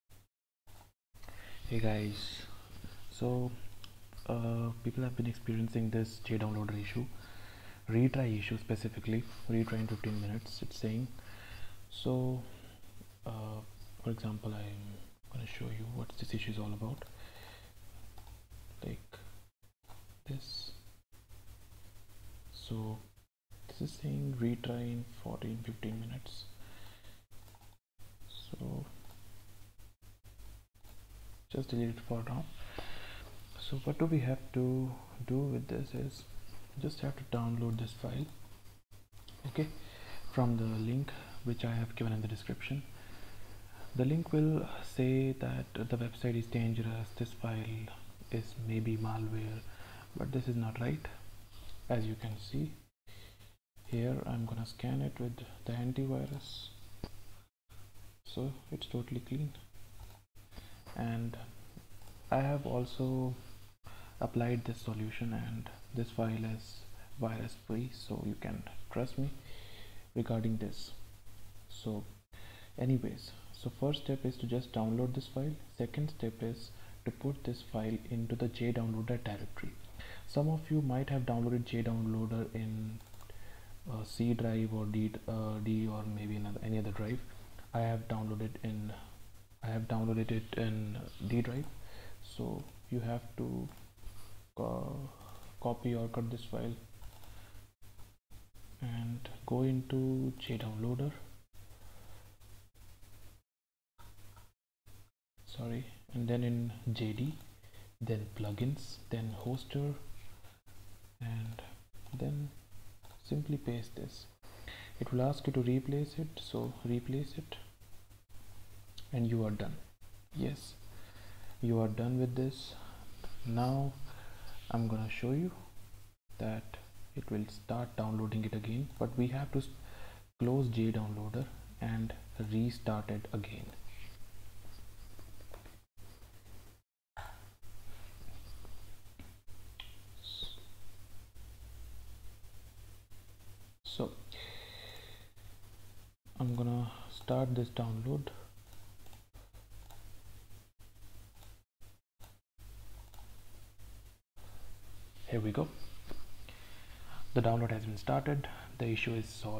Jdownloader2 retry in 15minutes issue [Solved]
In this video i have given solution to an issue we have been experiencing with downloader 2 i.e. retry in 15 minutes.

Refer to comments for the new link!!